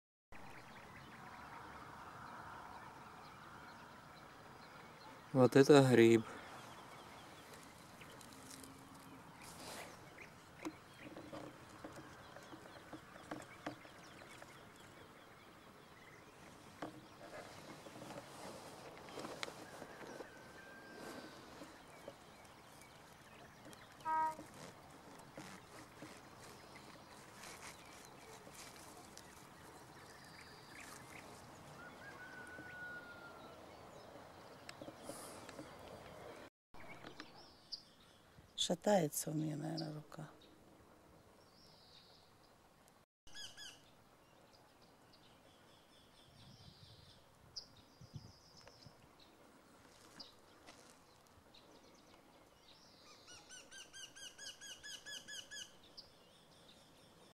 Сморчок обыкновенный.
Крупный гриб сморчок полевой вырос в дождик недалеко от дома. Вкусные грибы только зачастую с песочком до хруста на зубах.